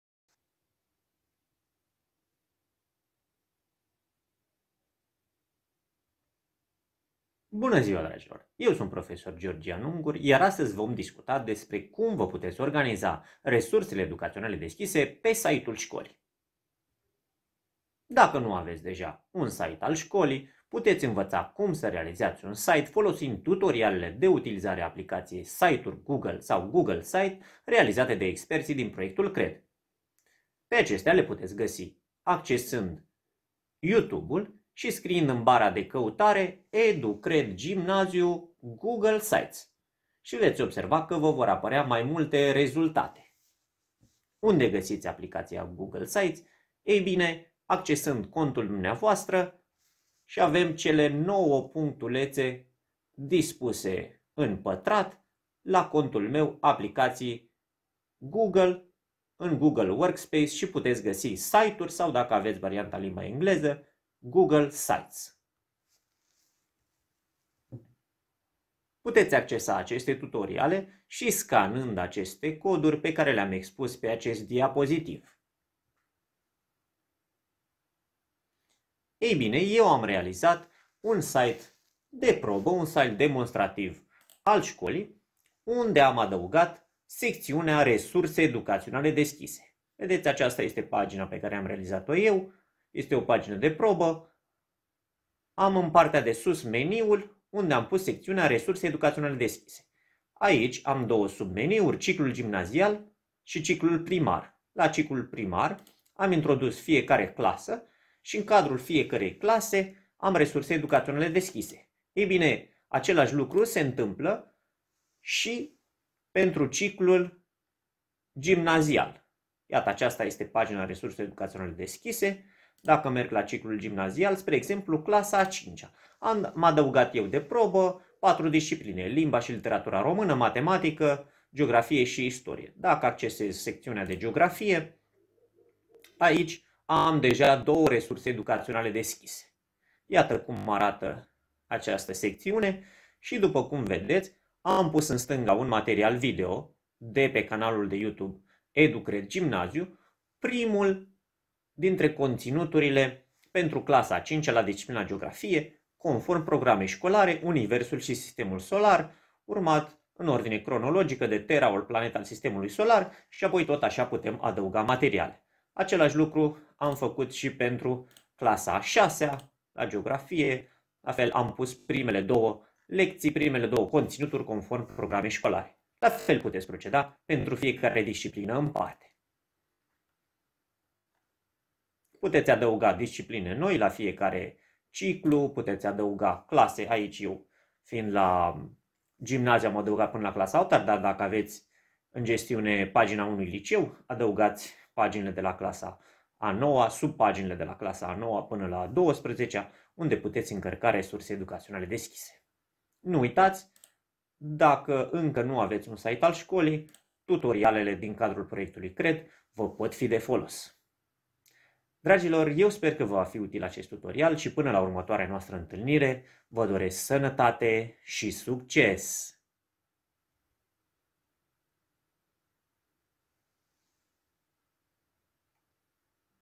Organizarea RED-urilor pe site-ul școlii
Bună ziua, dragilor!

     Această resursă educațională deschisă de tip video este un tutorial/ghid în care este exemplificat modul în care pot fi organizate resursele educaționale deschise pe site-ul unei instituții de învățământ.
     Pentru școlile care nu au încă un site web, este făcută trimiterea către o serie de tutoriale de lucru aplicația în Site-uri Google (Google Sites), cu ajutorul cărora pot realiza un site web.

Aplicații/programe utilizate: Microsoft Power Point, Site-uri Google (Google Sites)
Aplicație înregistrare ecran: ZDSOFT Screen  Recorder
Elemente grafice din biblioteca Microsoft Office 2019.

     Resursa educațională se adresează cadrelor didactice care doresc să publice și să organizeze resursele educaționale deschise proprii sau pe cele realizate în cadrul proiectului CRED, pe site-ul insituției de învățământ în care funcționează.

     Mulțumesc pentru atenție și vizionare!
     Sănătate și succes!